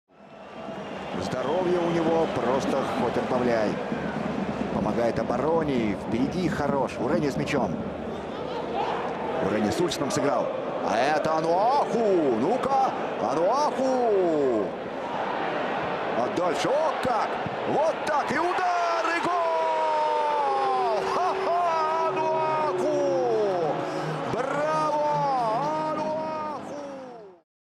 Paul Onuachu Goal FC Midtjylland 2-1 Manchester United UEFA Europa League 18/02/16 HD 720p
Watch in High Definition (HD) 720p, enjoy!
Paul Onuachu Goal FC Midtjylland 2-1 Manchester United UEFA Europa League 18/02/16